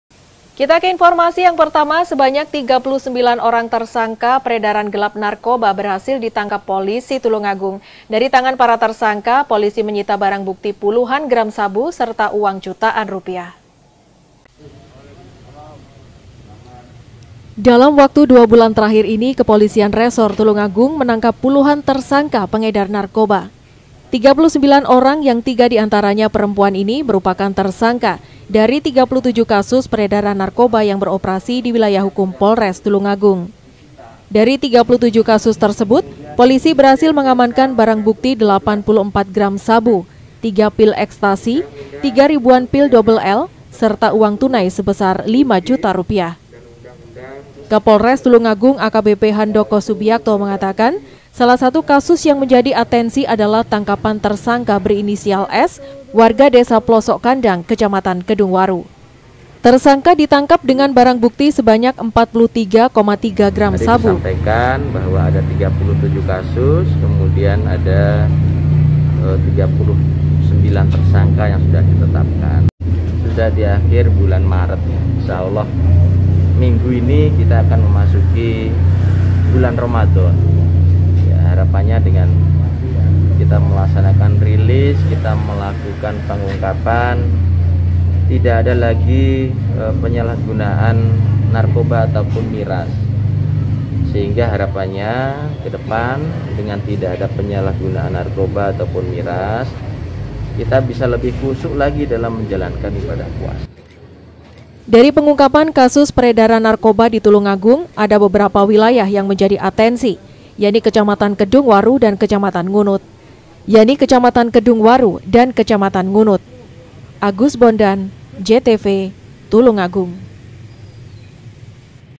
TULUNGAGUNG - 39 Tersangka Peredaran Narkoba Ditangkap Polisi
Sebanyak 39 orang tersangka peredaran gelap narkoba berhasil ditangkap polisi tulungagung.dari tangan para tersangka polisi menyita barang bukti puluhan gram sabu/ serta uang jutaan rupiah.